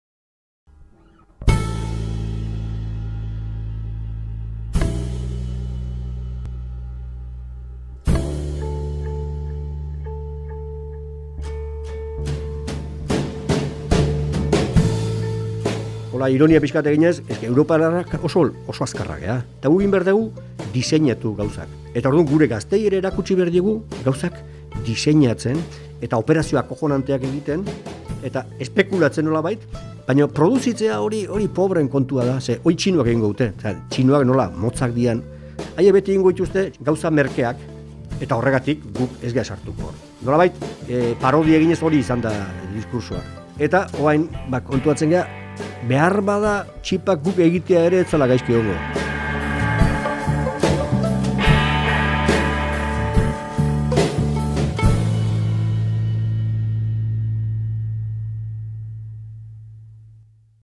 PODCAST #58 (teaserra) | Auto fabrika gelditu behar dugu, COVIDagatik eta Taiwaneko lehorteagatik
Urtarril amaieran Gasteizko Mercedes konpainiako zuzendaritza langileen ordezkariekin bildu zen ohartarazteko fabrikako ekoizpen-katea eten beharko zutela autoentzako hainbat pieza elektroniko eskuratu ezinik zebiltzalako –zehazki, txip elektronikoak–.  Bi hilabete beranduago, martxoaren 27an produkzioa eten behar izan du. Gasteizkoa aitzakia hartuta, mikrotxipen krisiak automozioaren indusitrian izan duen eragina aztertu dugu Pello Zubiriarekin.